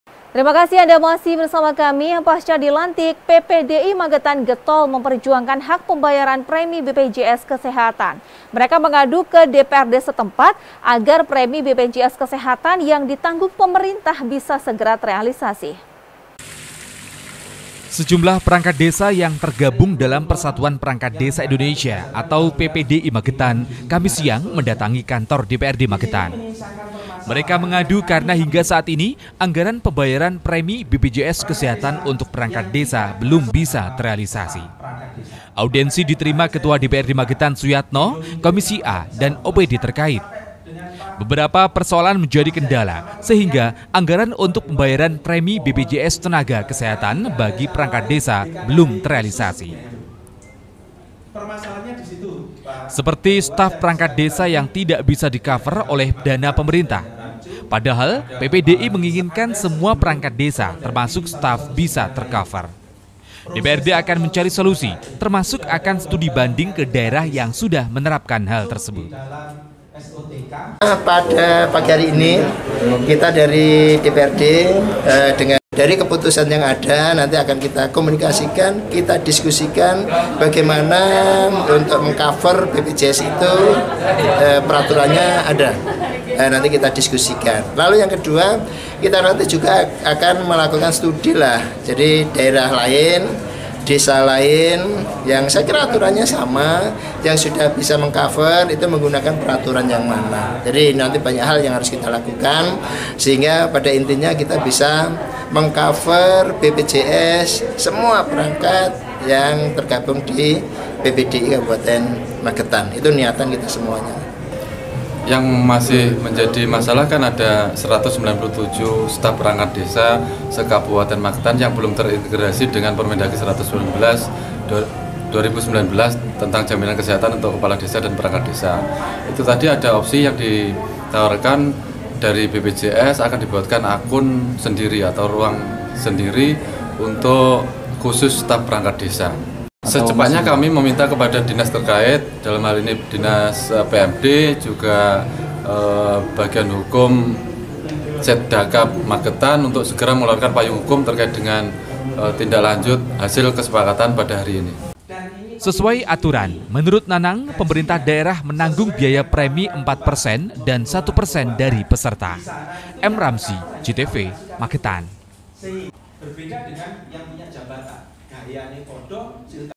MAGETAN - Perangkat Desa Wadul Dewan, Soal BPJS Kesehatan
Pasca dilantik, PPDI Magetan getol memperjuangkan hak pembayaran premi BPJS kesehatan. Mereka mengadu ke DPRD setempat agar premi BPJS kesehatan yang ditanggung pemerintah bisa segera terealisasi.